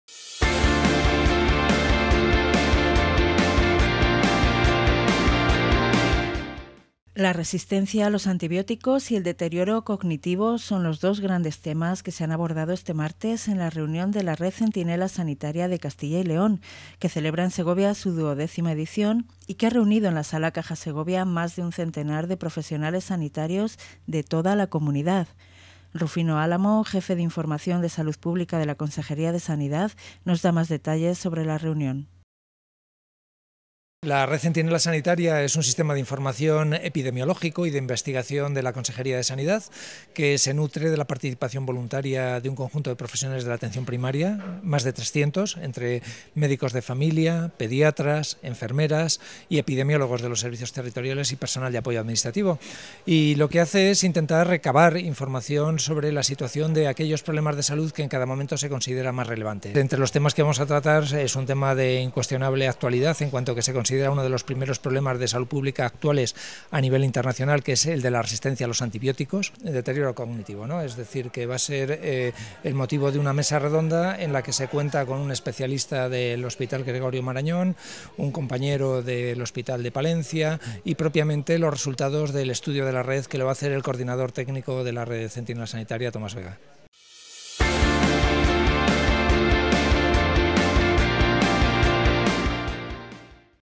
La resistencia a los antibióticos centra la Reunión de la Red Centinela Sanitaria
La resistencia a los antibióticos y el deterioro cognitivo son los dos grandes temas que se han abordado este martes en la reunión de la Red Centinela Sanitaria de Castilla y León, que celebra en Segovia su duodécima edición y que ha reunido en la Sala Caja Segovia a más de un centenar de profesionales sanitarios de toda la Comunidad que voluntariamente participan en este sistema de vigilancia e investigación de la salud.